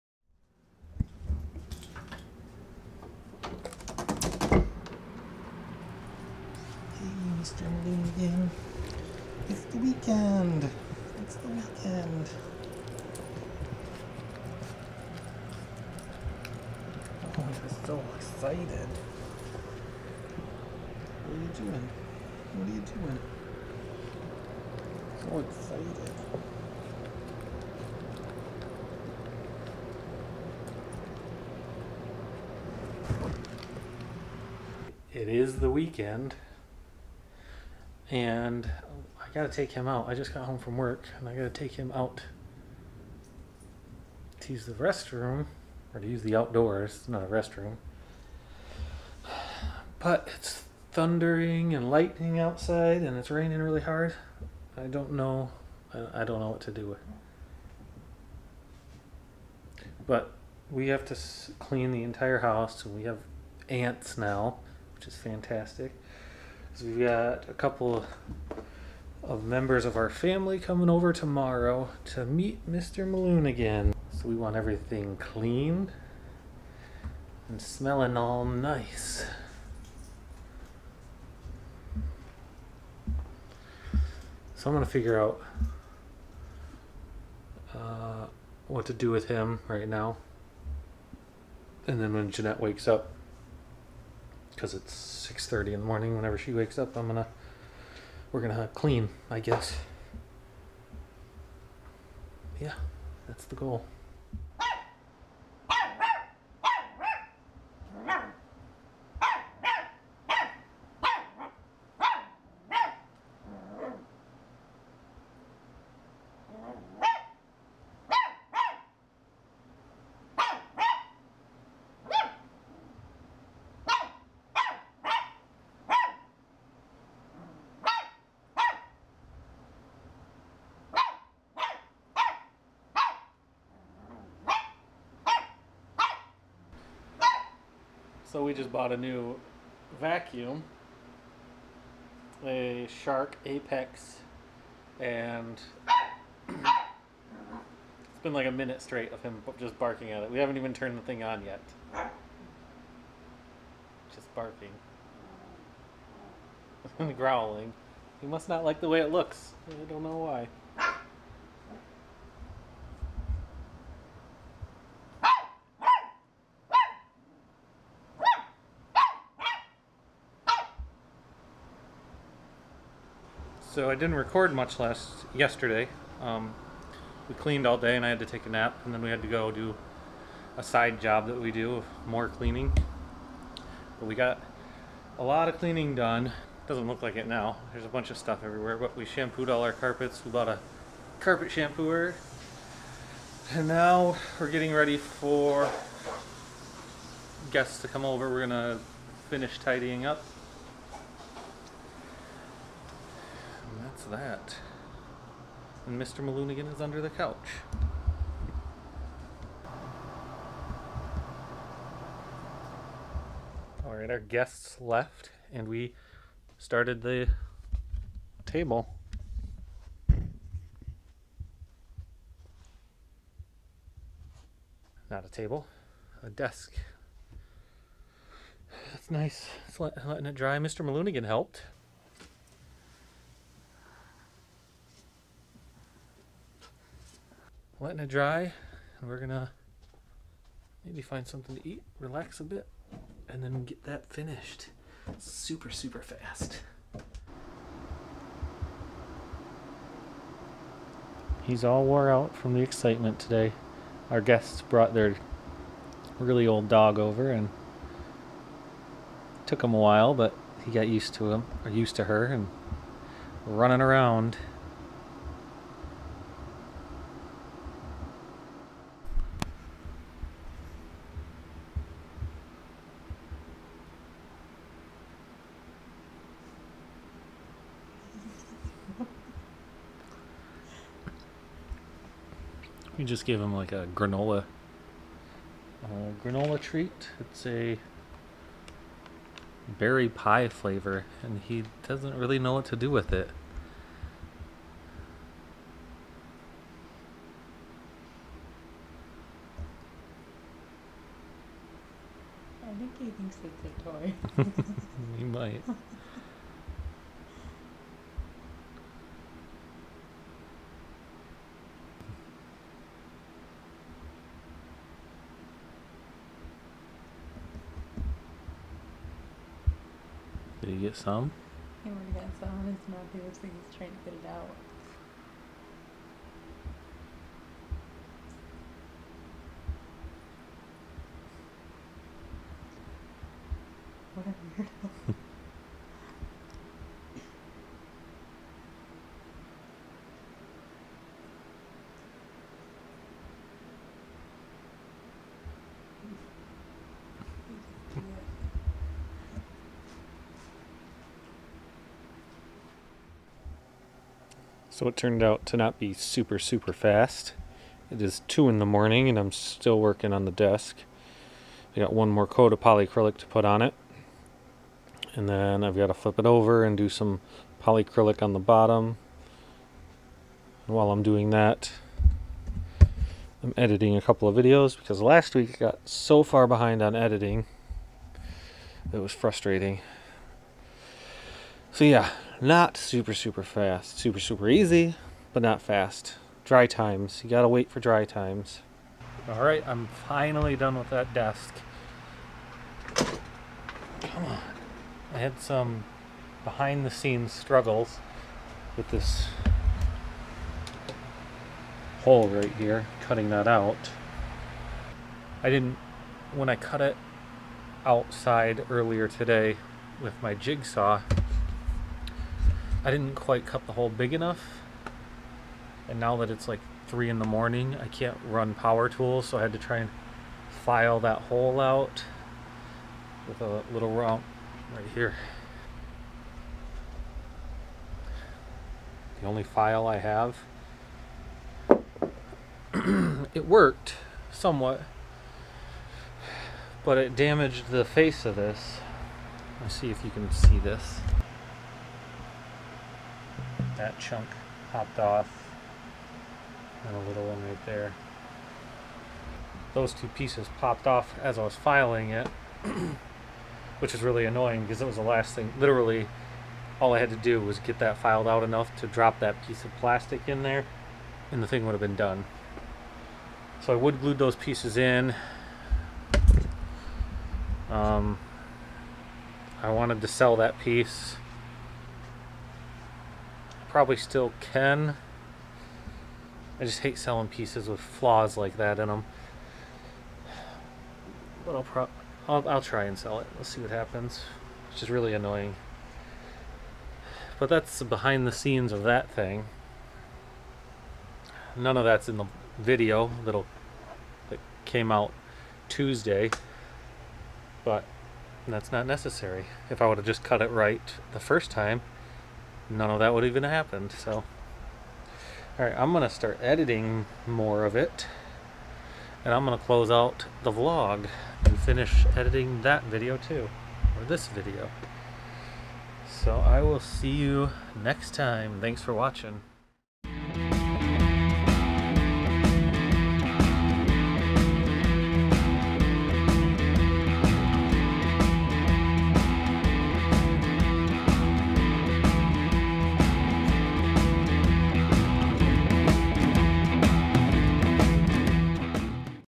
Vlog Ep. 39: Shih Tzu Puppy Doesn't Like Our New Vacuum
We clean for guests. We work on a project and Mr. Maloonigan doesn't like our new vacuum.